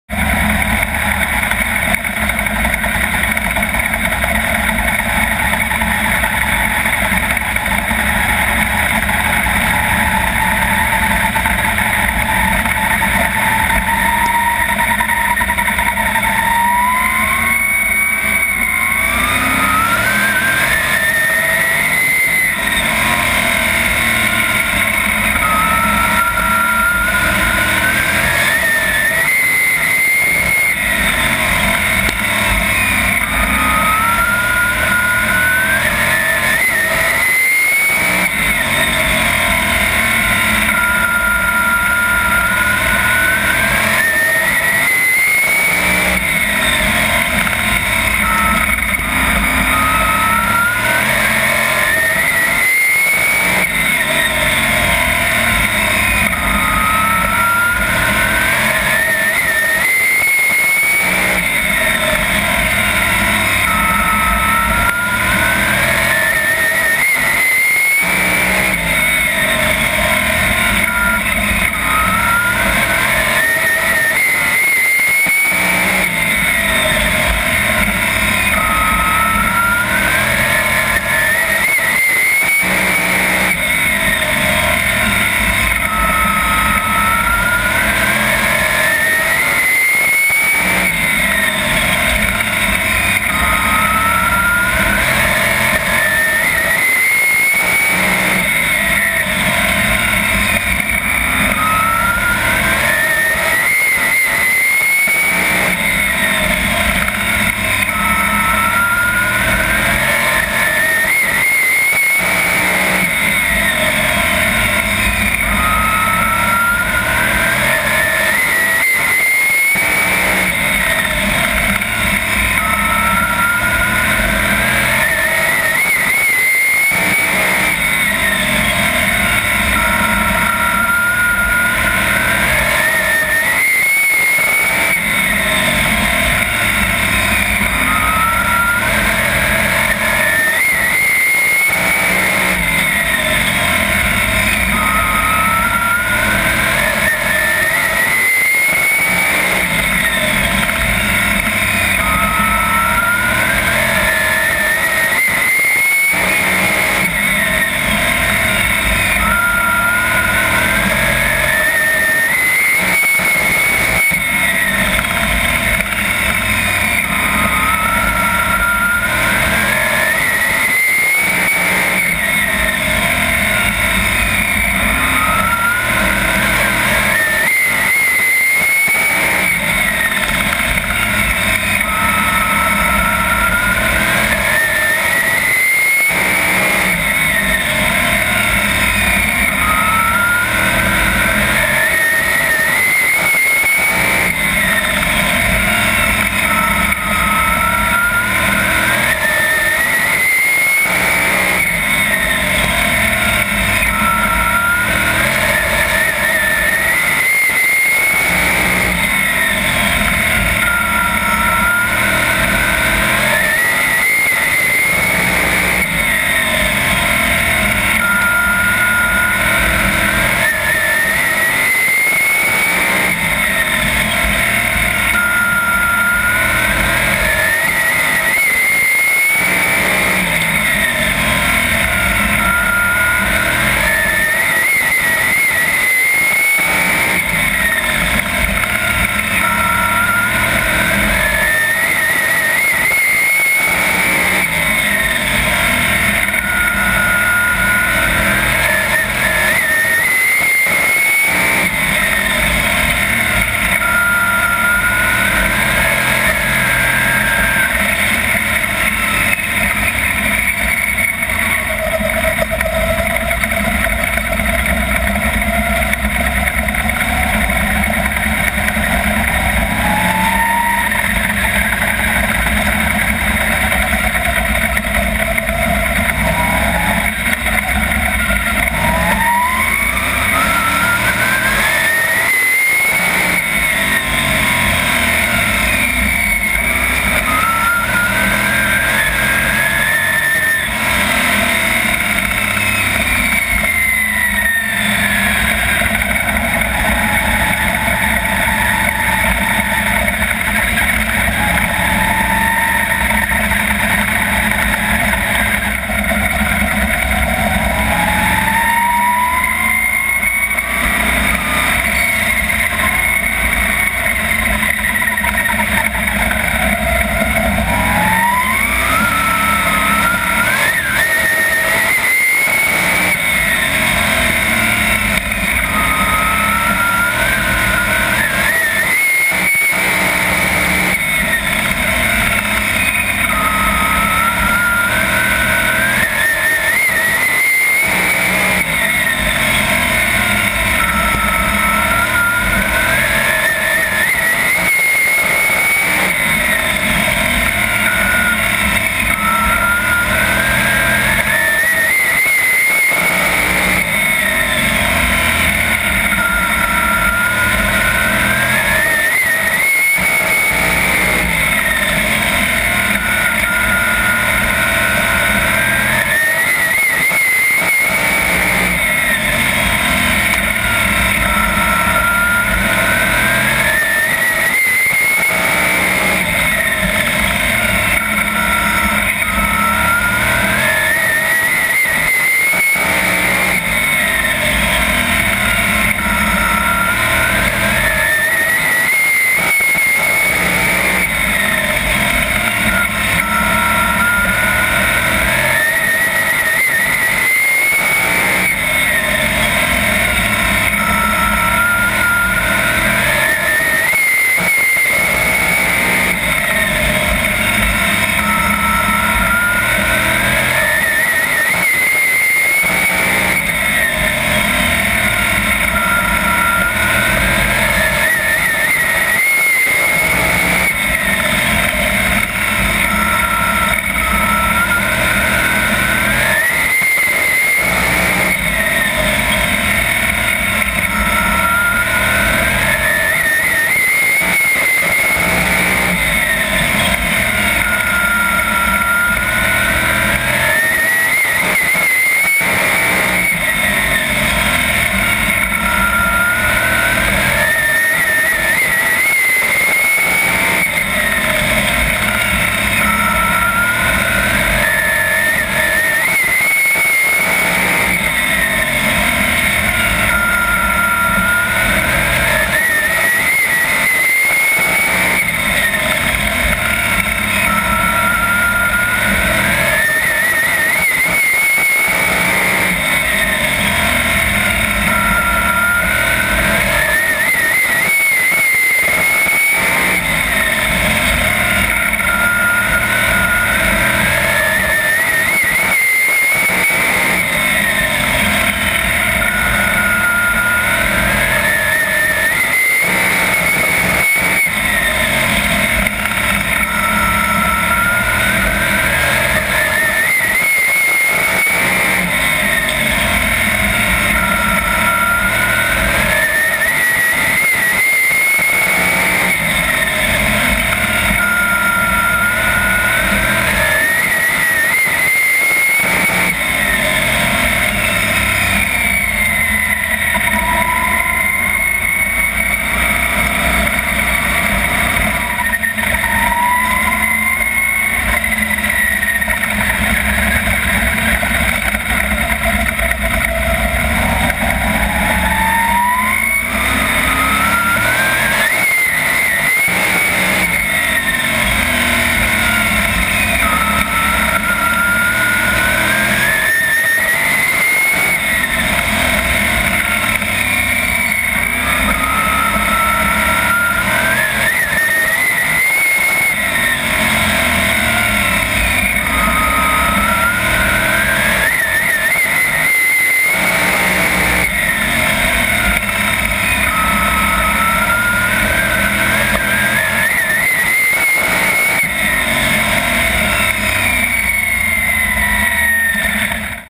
★ Derek Snyder GoPro HD on board @ Owosso Speedway 410 Sprint Car 9-25-11 ★
Derek Snyder rocks his GoPro Motorsports HD on board camera on the Wayne Stickney owned 410 pavement Sprint Car at Owosso Speedway. Snyder starts 14th and finishes 7th. Joe Swanson wins it.